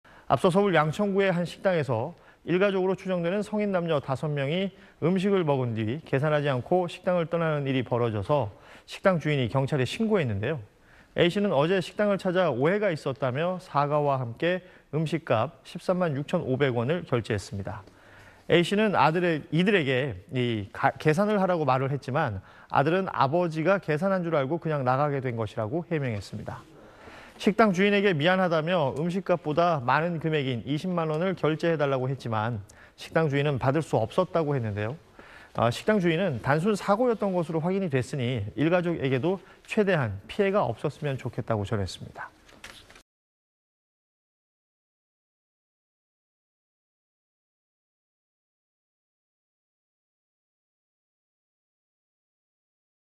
"오해가 있었다"…'먹튀 논란' 일가족, 식당 찾아 사과 / SBS / 뉴스딱
식당에서 음식값을 내지 않고 나가서 이른바 먹튀 논란이 제기된 일가족이 오해가 있었다며 식당을 방문해 사과했습니다.

앞서 서울 양천구의 한 식당에서 일가족으로 추정되는 성인 남녀 5명이 음식을 먹은 뒤 계산하지 않고 식당을 떠나는 일이 벌어져서 식당 주인이 경찰에 신고했는데요.

A 씨는 어제(26일) 식당을 찾아 "오해가 있었다"며 사과와 함께 음식값 13만 6천500원을 결제했습니다.

A 씨는 아들에게 계산을 하라고 말했지만 아들은 아버지가 계산한 줄 알고 그냥 나가게 된 것이라고 해명했습니다.

식당 주인에게 미안하다며 음식값보다 많은 금액인 20만 원을 결제해달라고 했지만 식당 주인은 받을 수 없었다고 했다는데요.

식당 주인은 "'단순 사고'였던 것으로 확인 됐으니 일가족에게도 최대한 피해가 없었으면 좋겠다"고 전했습니다.

(화면출처 : 국민일보)